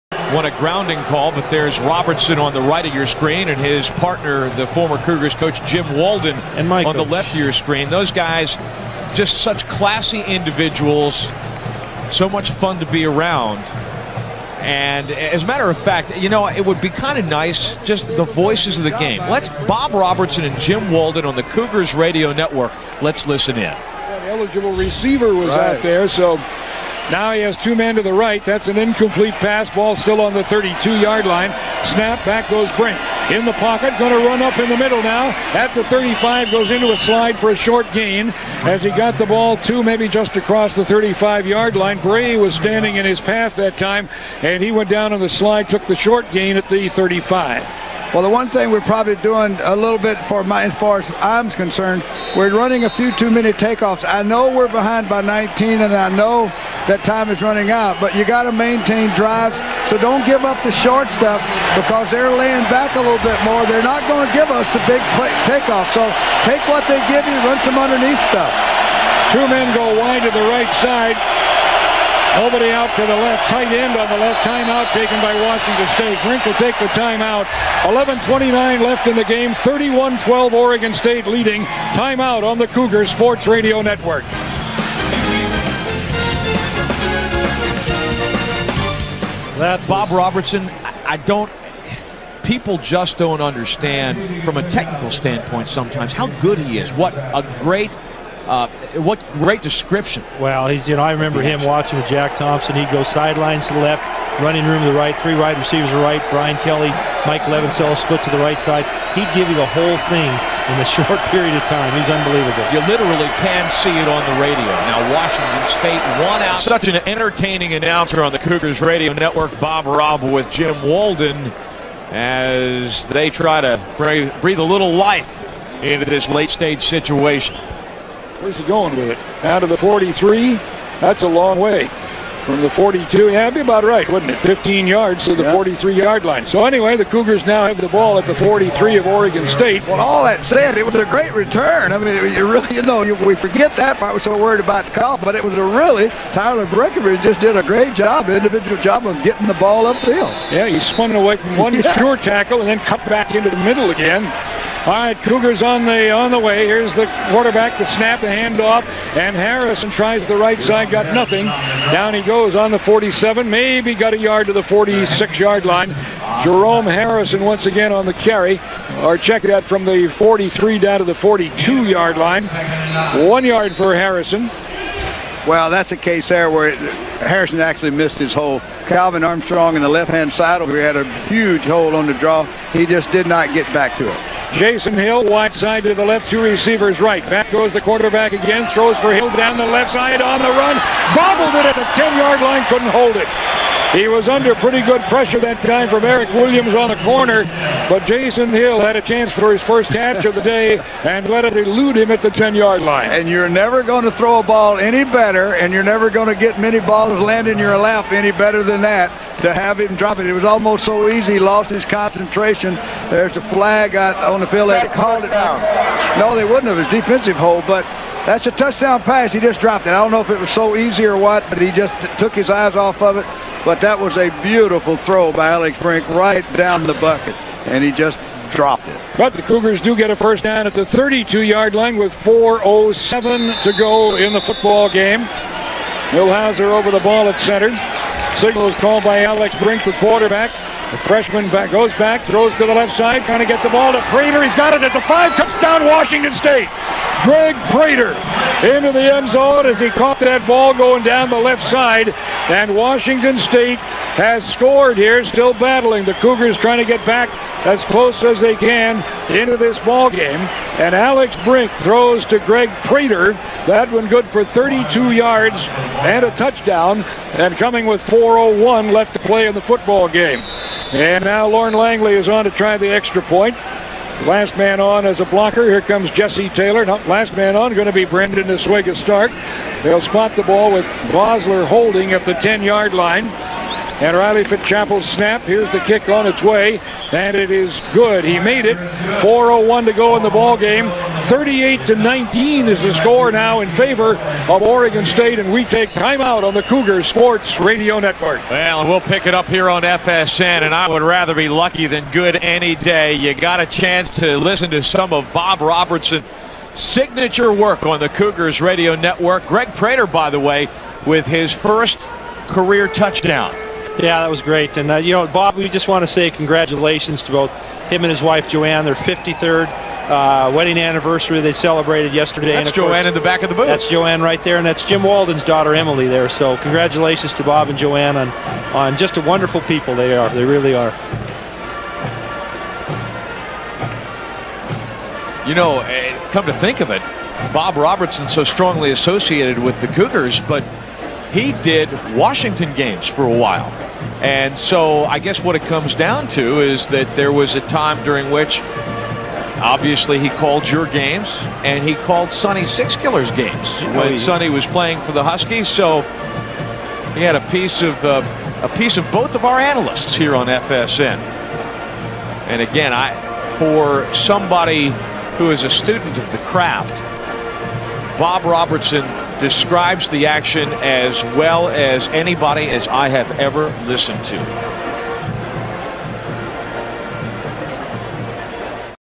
Bob Robertson Calls WSU Football Live on Fox Sports Northwest
"Always be a good sport, be a good sport all ways." - Bob Robertson

Game Played: 10/23/2004